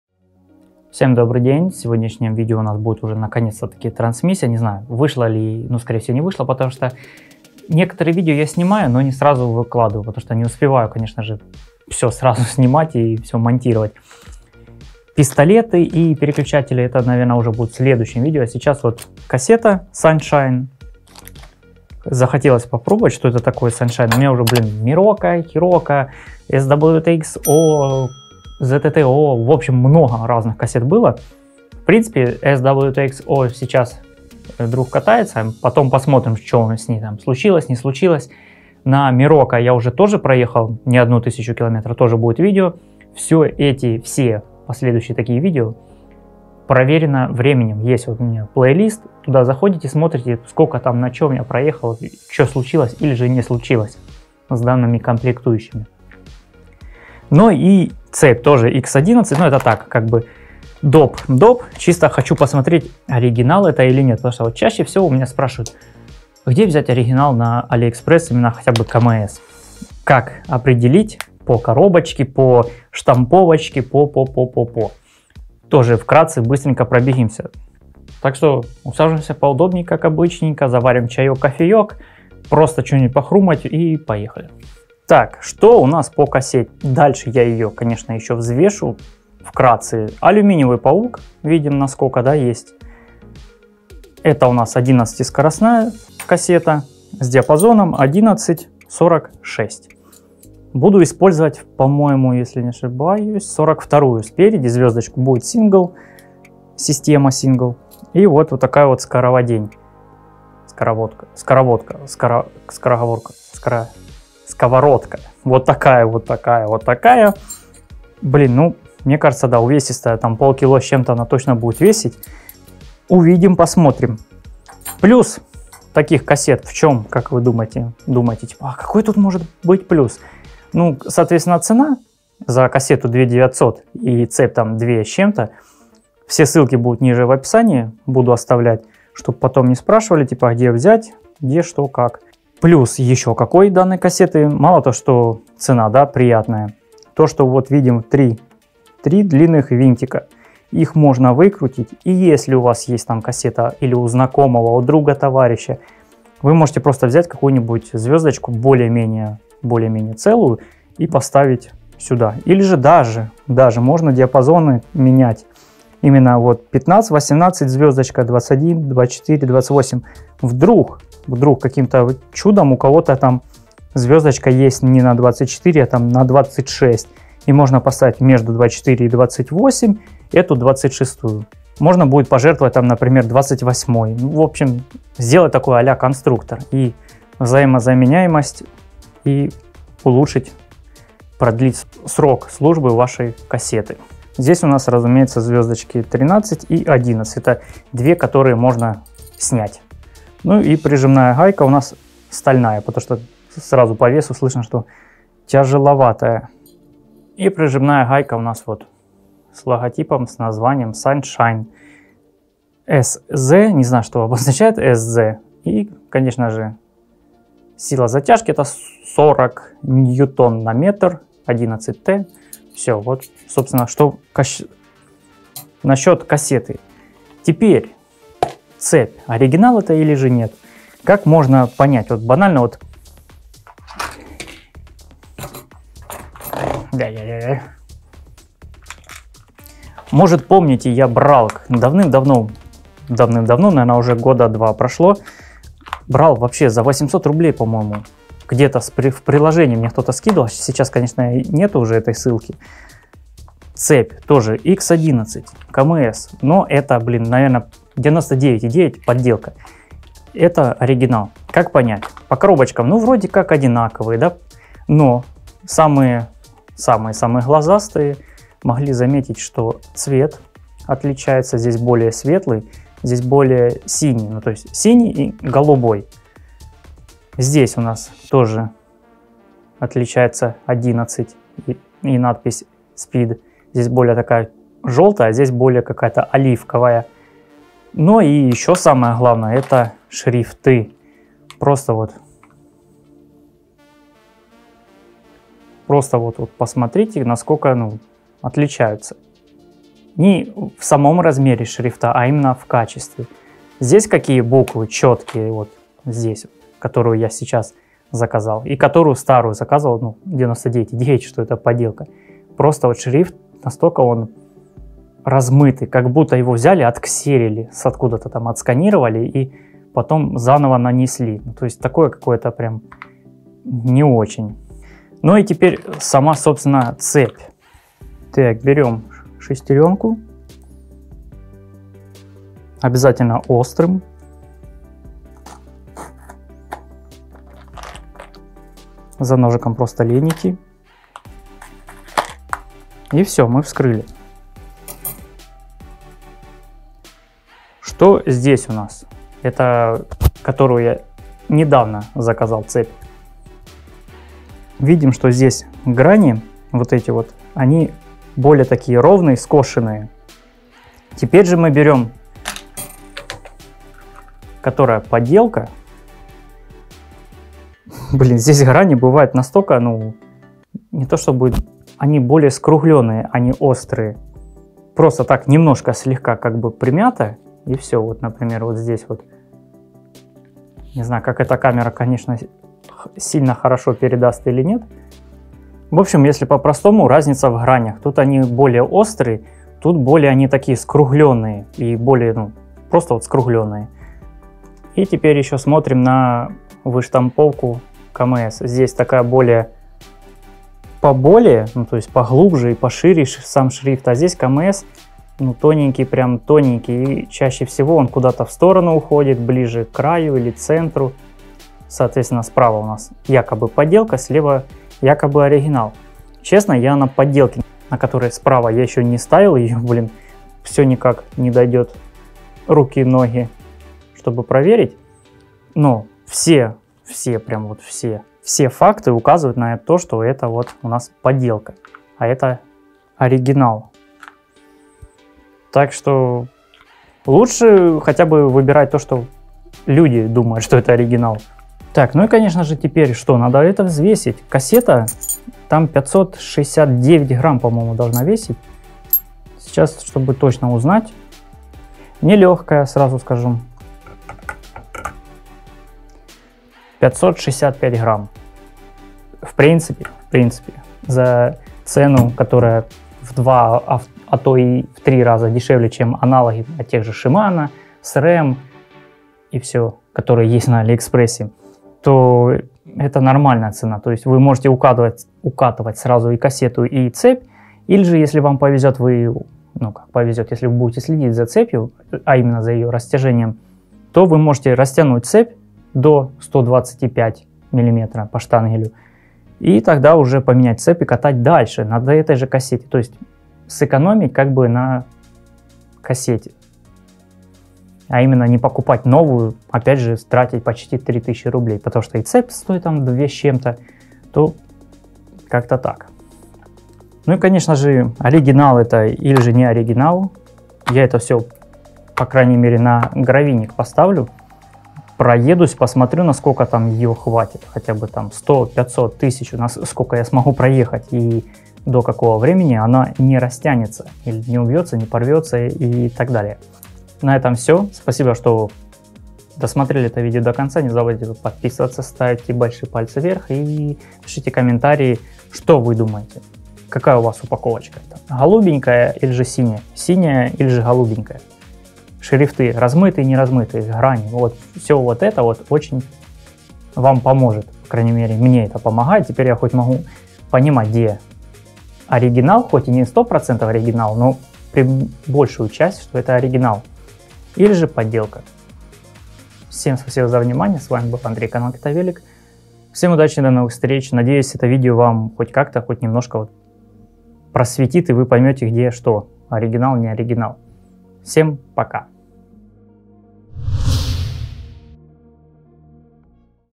11 скоростная кассета Sunshine 11-46 и оригинальная цепь Kmc x11 с Aliexpress
В этом видео посмотрим 11 скоростная кассета Sunshine 11-46 и оригинальная цепь Kmc x11 с Aliexpress.